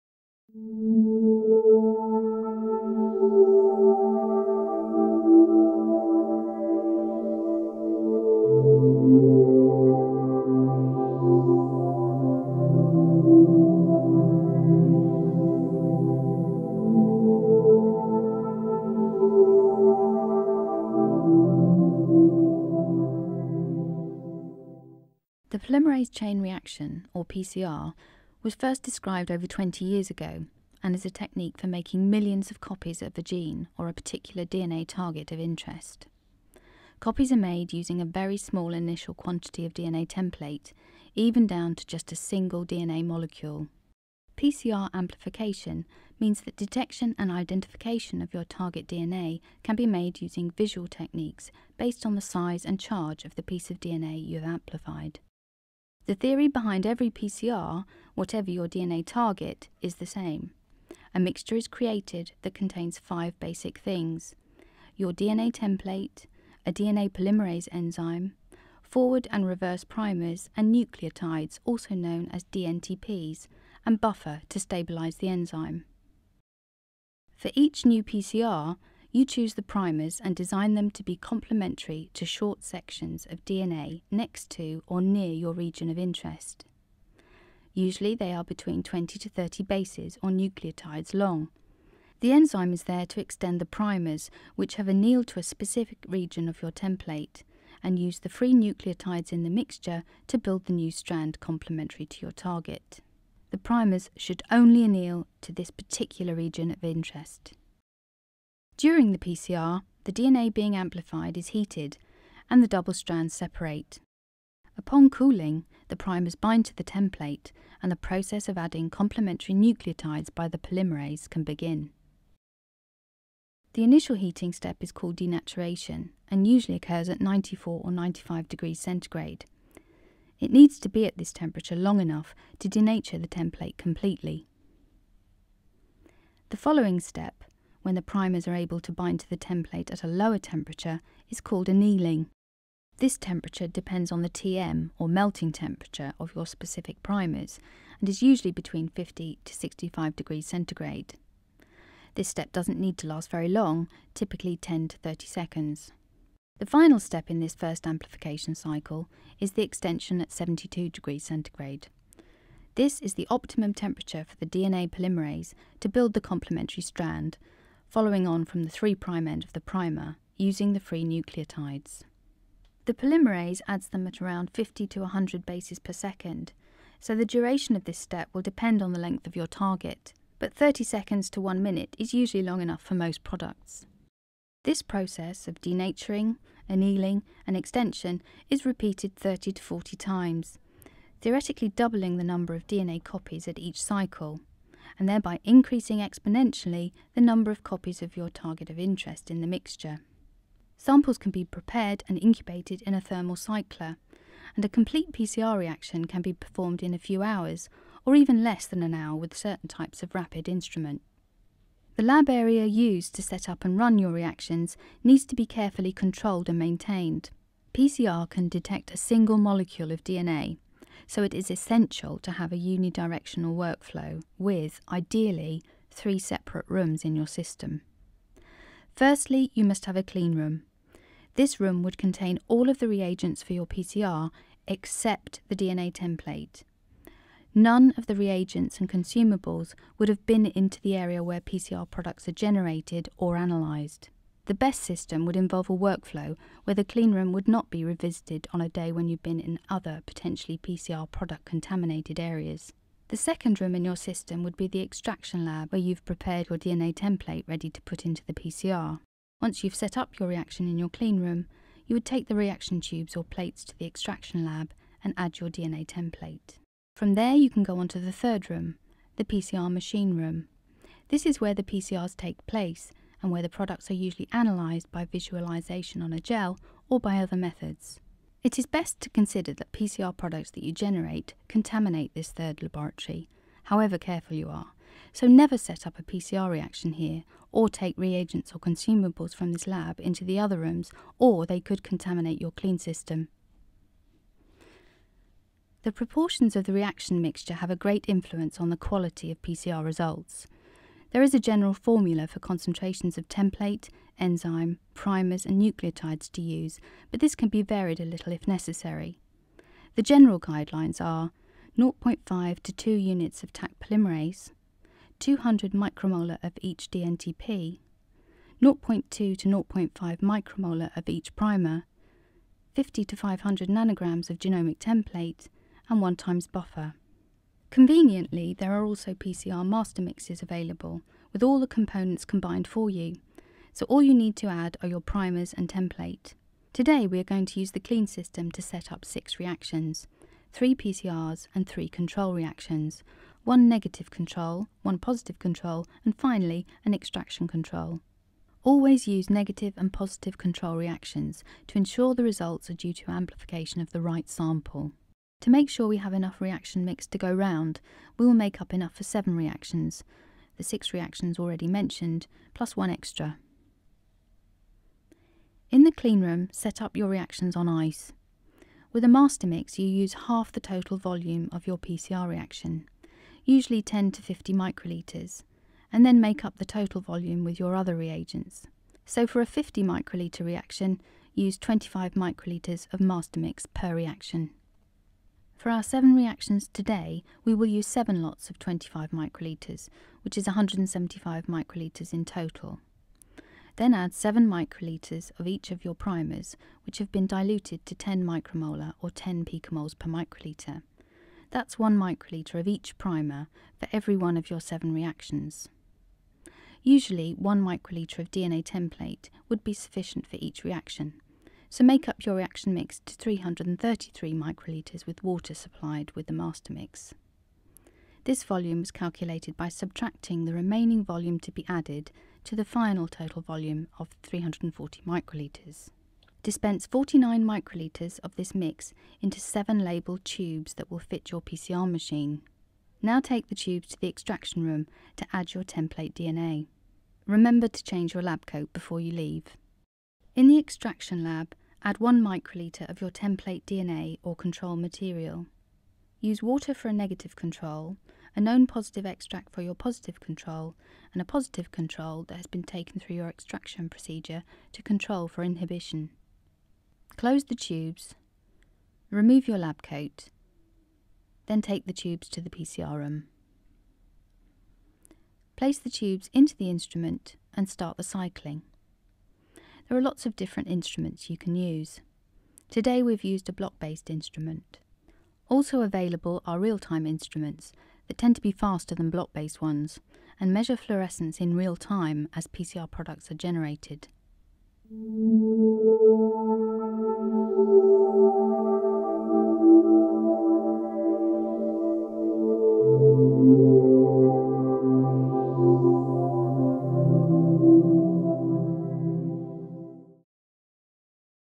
How to set up a PCR
In this film, Dr Cath Arnold from the Health Protection Agency demonstrates how to set up a Polymerase Chain Reaction (PCR).
For a transcript of this film, paste the below link into your browser: